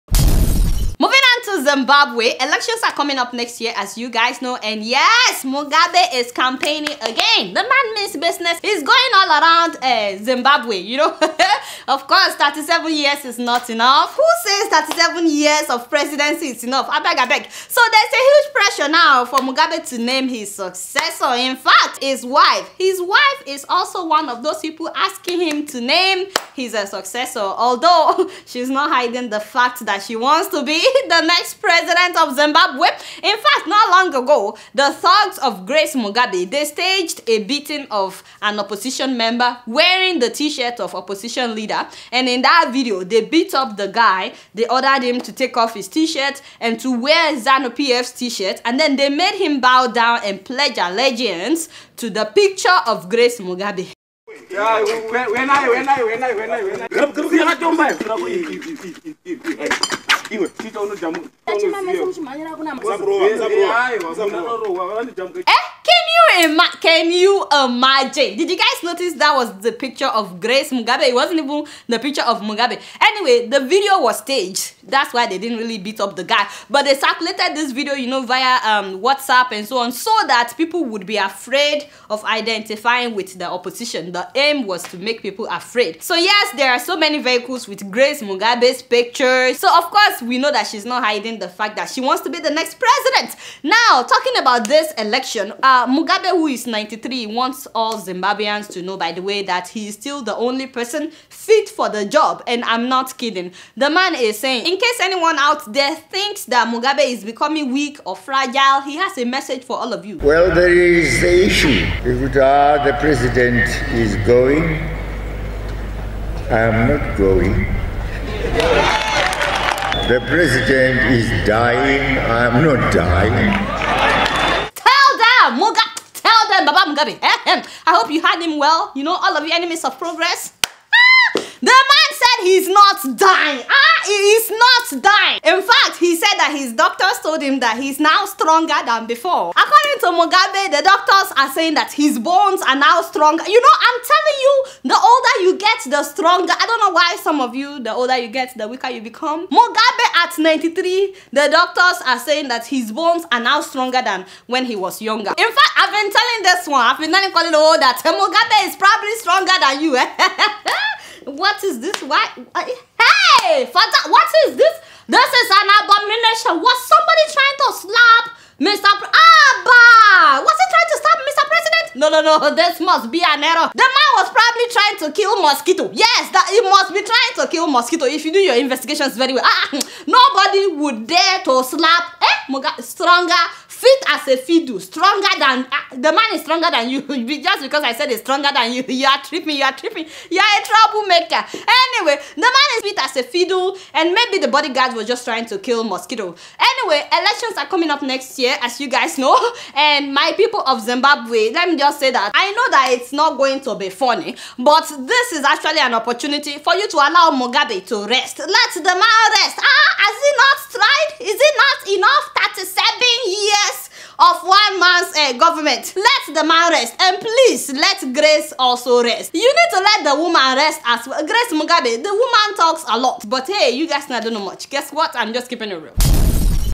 "I'm Not Dying Or Leaving," 93-Year-Old Zimbabwean President Robert Mugabe
93-year-old Robert Mugabe of Zimbabwe says he is not dying or leaving.